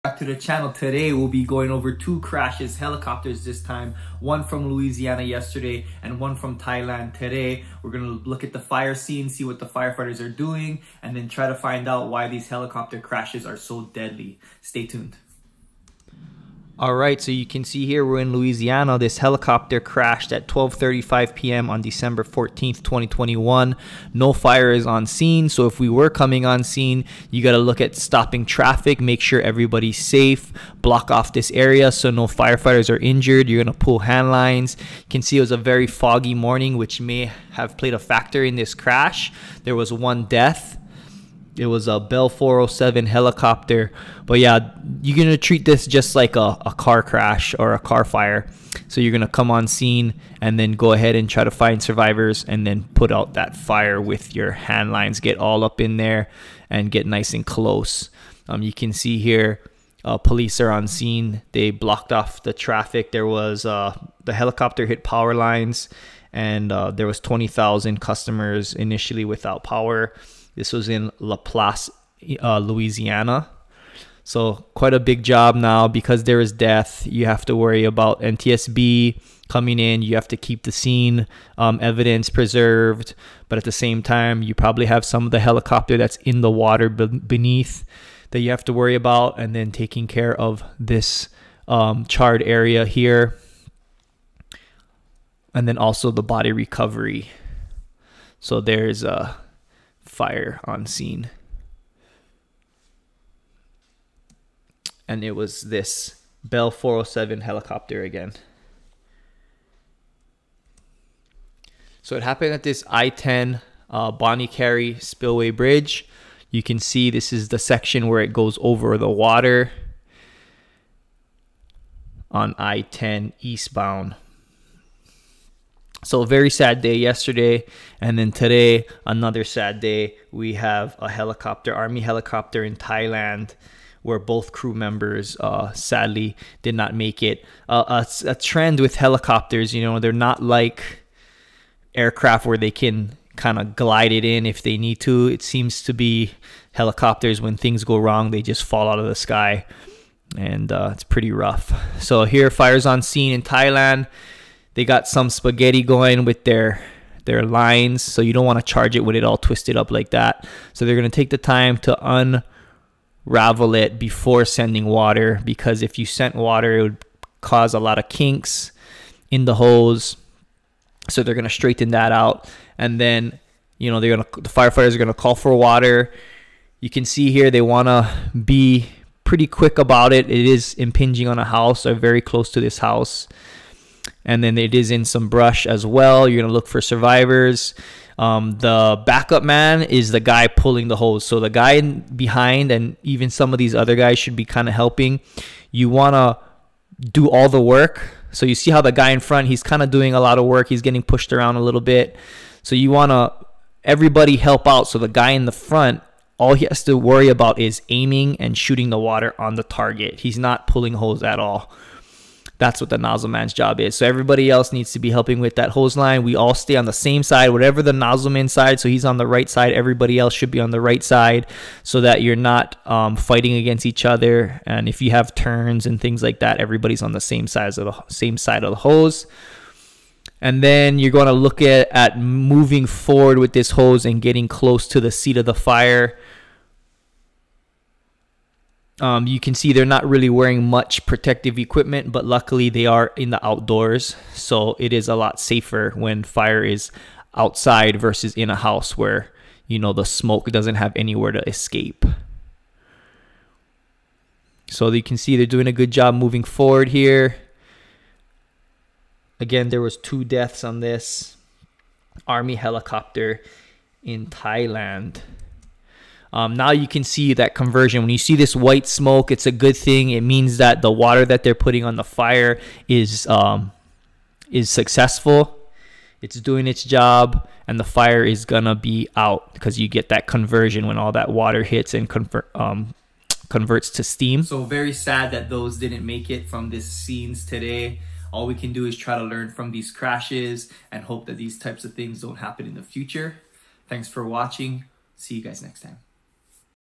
Helicopter crash in Louisiana and Thailand
A Bell 407 helicopter crashes on Bonnie Carre Bridge in Louisianna on December 14, 2021 and also a Enstrom 480B Thailand Army Helicopter crashes today at 9:40am, no survivors on either crash.